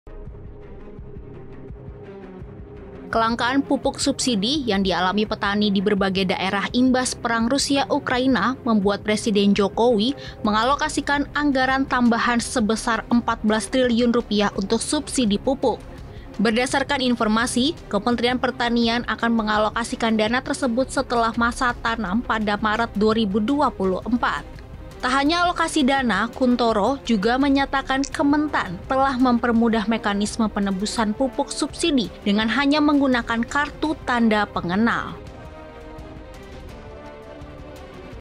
Headline - Dana Tambahan Rp.14 T untuk Subsidi Pupuk | JURNAL 19
Presiden Jokowi telah mengalokasian dana tambahan sebesar 14 triliun rupiah untuk pupuk bersubsidi. Dana ini dialokasikan guna menanggapi keluhan petani, mengenai langkanya pupuk subsidi.